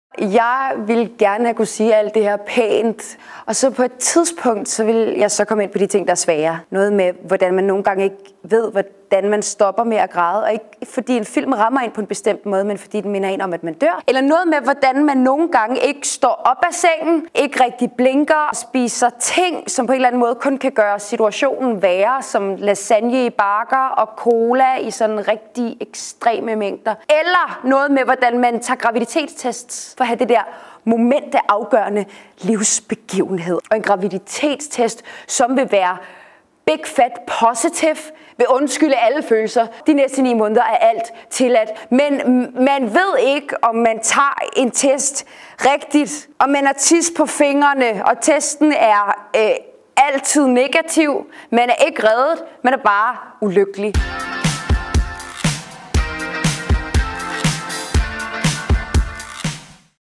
Manifest for skrøbelige autoriteter - tis på fingrene
Aarhus Teaters nye huskunstner, Nanna Cecilie Bang, har skrevet Manifest for skrøbelige autoriteter, som er en punkpoetisk tekst for tre skuespillere. En efter en snakker de, til de besvimer, og den næste tager over. Indtil der ikke er flere ord tilbage, kun det sårbare og lidt for skamløst ærlige menneske. Hør Nanna Cecilie Bang, som udover at have skrevet teksten også instruerer fortælle om Manifest for skrøbelige autoriteter.

Forestillingen spiller på Studio på Aarhus Teater fra 12. september.

TEKST OG ISCENESÆTTELSE NANNA CECILIE BANG
SCENOGRAFI DAVID GEHRT
LYSDESIGN KIM GLUD & MIKKEL OVE HANSEN
LYDDESIGN ANDREAS A. CARLSEN
MEDVIRKENDE: METTE KLAKSTEIN WIBERG, DURITA DAHL ANDREASSEN, CAMILLA LAU